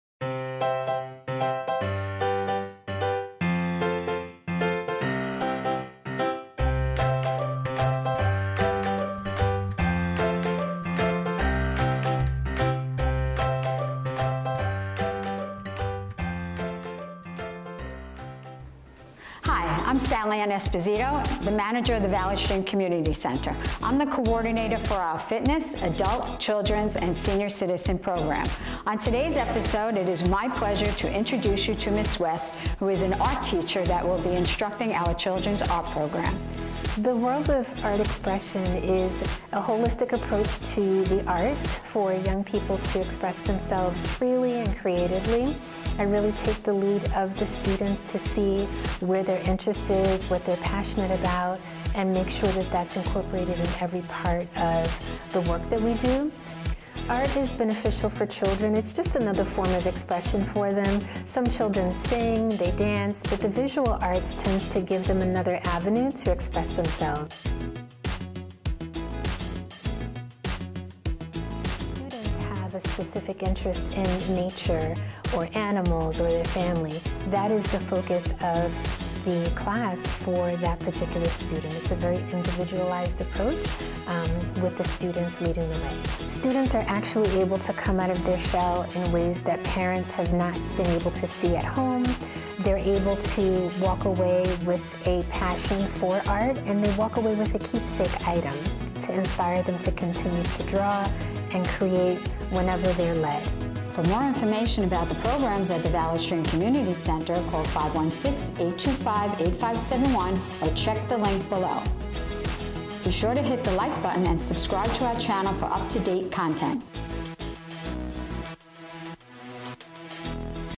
Valley Stream Road Show Season 1 Episode 15, Children's Art Program
Take a look at our unique children's art program taught by our wonderful instructor Chotsani Williams West!